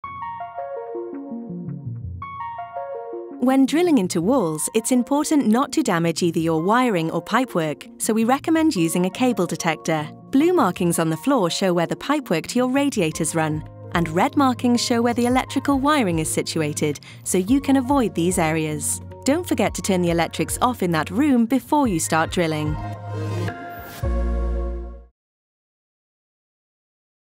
Wiring and waterpipe zones
Watch our home demonstration video all about the new wiring and waterpipe zones in your Persona Home.